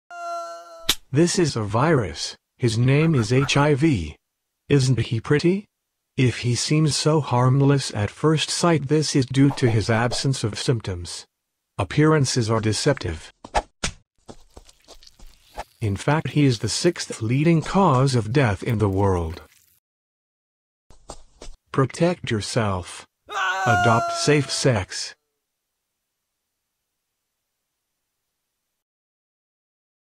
Awareness Campaign HIV/AIDS - Adopt Safe Sex (Concept)
It's only a concept for an English course at ECE Paris, What's important ?